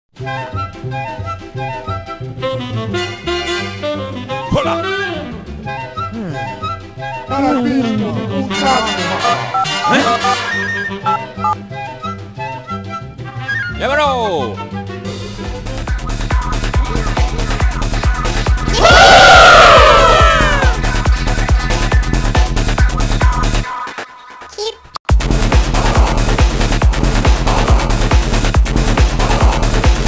Mole Rave
Some European cell phone company commercial. :3 If you know what country, tell me.